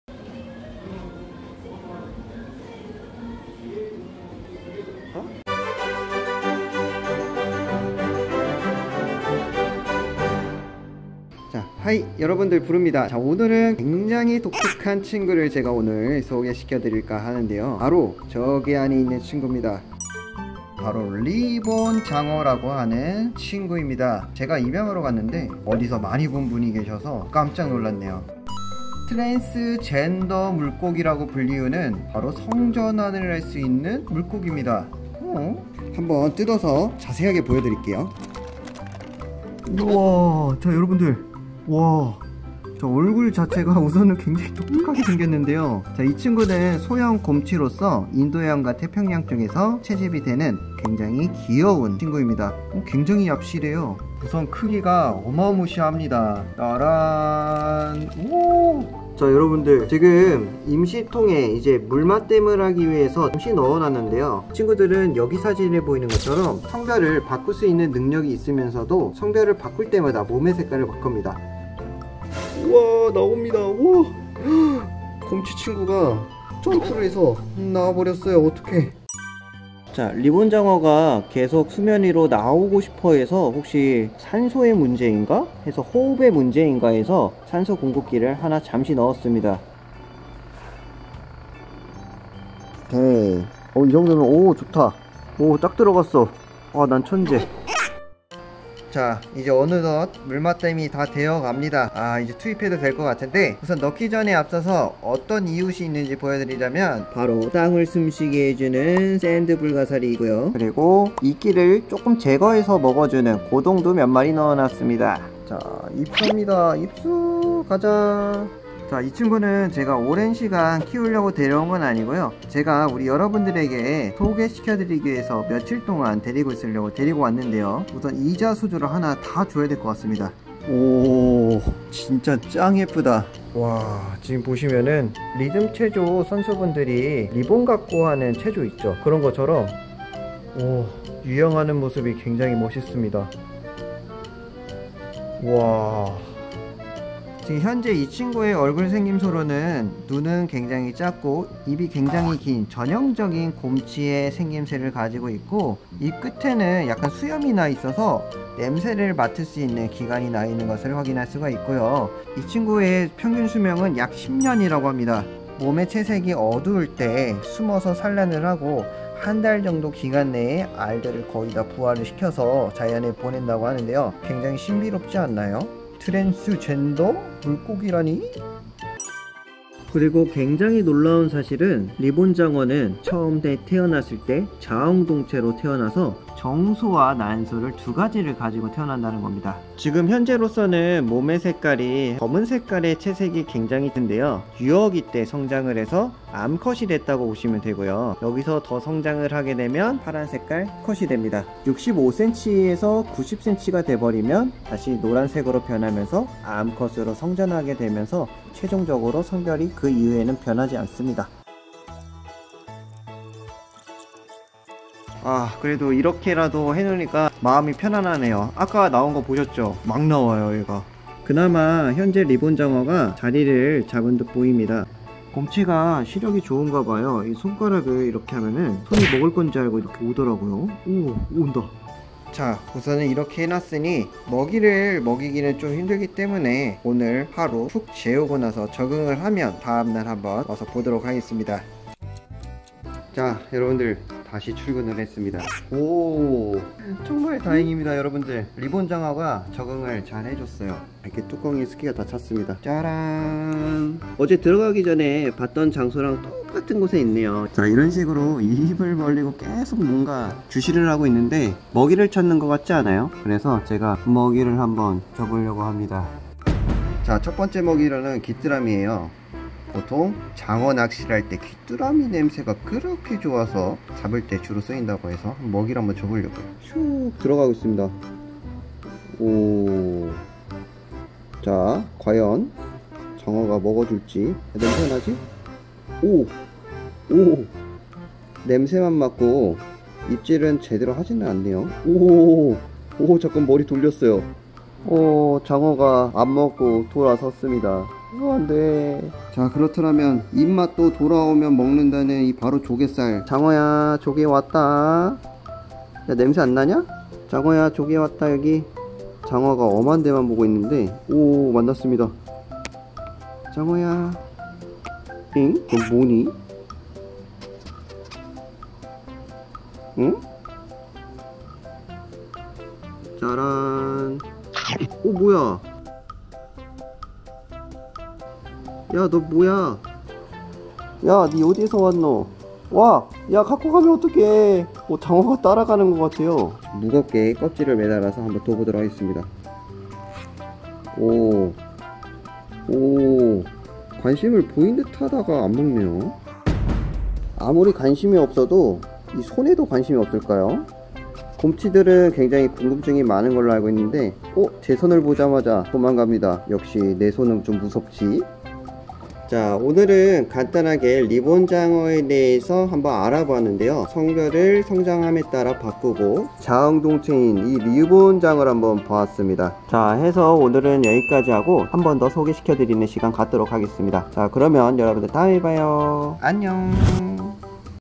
신기함주의)성전환3번이나 하는 트렌스젠더 물고기라니...?
스스로 성전환하는 생물들은 꽤 많습니다.
흰둥가리부터 강성돔.. 그리고 자웅동체도 많지요.

신기했다면 좋아요~꾸욱~~